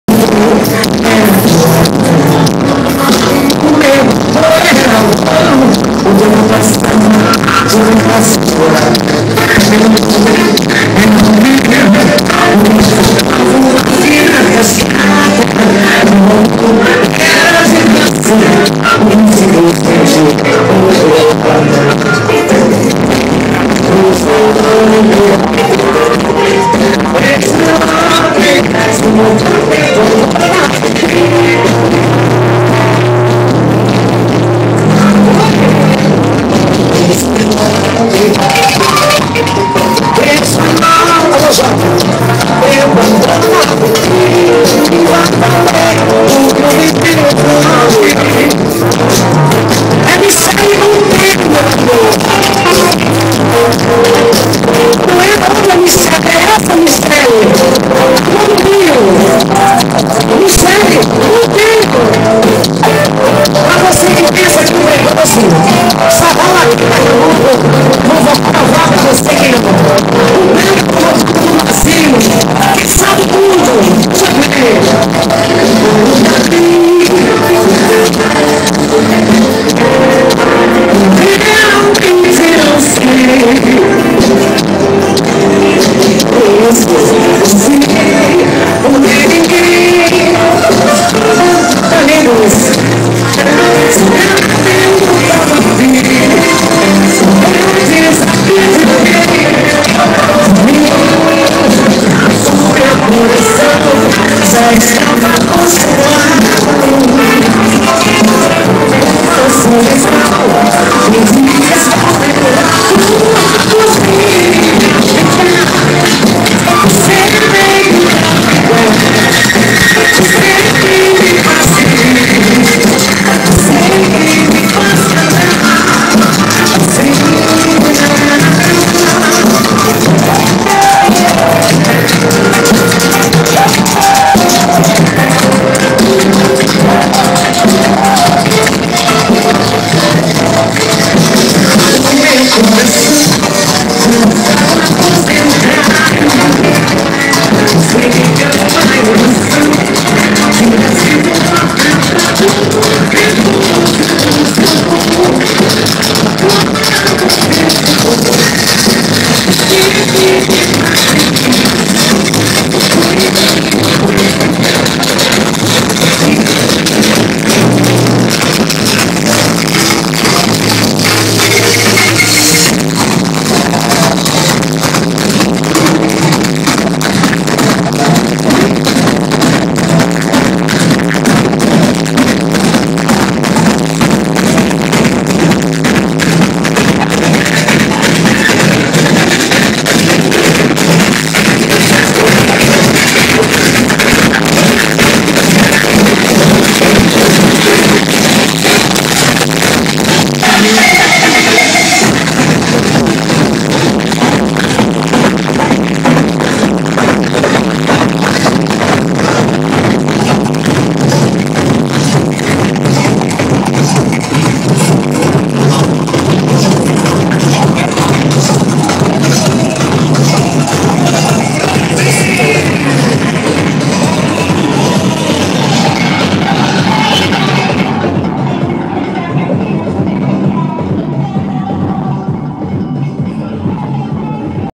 11ª Parada da Diversidade de Recife / Pernambuco
Avenida Boa Viagem, Recife, 16/09/2012. A 11ª edição do evento tem como tema Democracia em Todos os Cantos: Vamos Cantar um Pernambuco sem Homofobia.